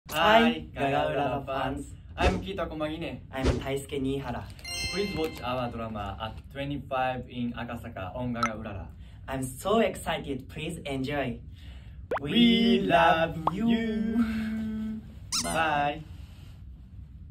The actors of "At 2500, in Akasaka" have a special message for GagaOOLala fans! 😍💜
When Sirosaki, an underdog stage actor, gets cast in a gay drama series, he is surprised to learn that he will be working with Hayama, his charming clubmate from university who is now a popular actor. During production, Sirosaki realizes that he will need some inspiration and goes to find some where the local gay people hang out. What he didn't expect was to meet Hayama there, which eventually leads to a drunken kiss. He soon finds himself confused and conflicted about his feelings as he struggles to keep his emotions contained on-screen…

☆ GagaOOLala joins forces with TV Tokyo to co-produce a new manga-adapted BL drama!
☆ The original book sold millions of copies, and the director of "Naked Dining" and "My Strawberry Film" takes the helm of the live-action version.
☆ Taisuke Niihara from "Eternal Yesterday" and Tokusatsu star Kiita Komagine couple up and the lines between on-screen chemistry and off-screen relationships get blurry!